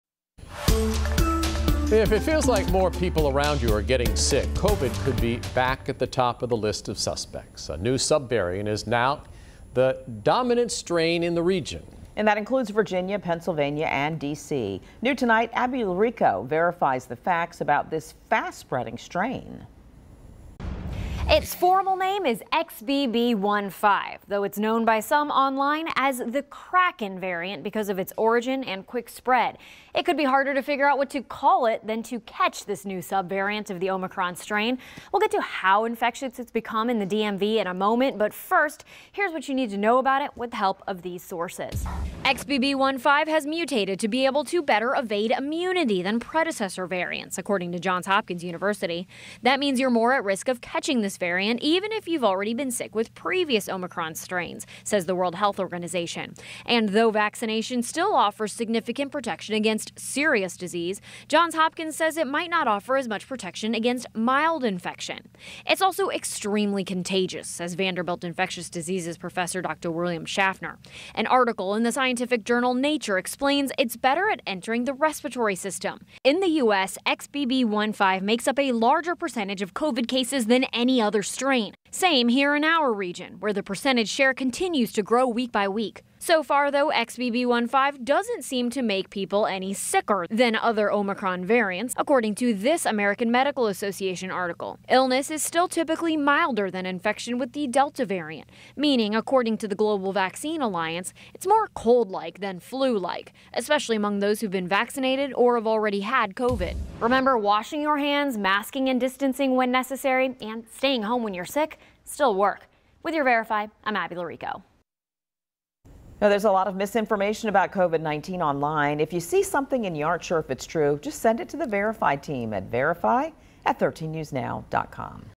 VERIFY the facts about new XBB.1.5 COVID-19 strain
If it seems like more people around you are getting sick, COVID could be back at the top of the list of suspects. A new subvariant is now the dominant strain in the region, which includes Virginia, Pennsylvania, and D.C. Abby Llorico verifies the facts about this fast-spreading strain.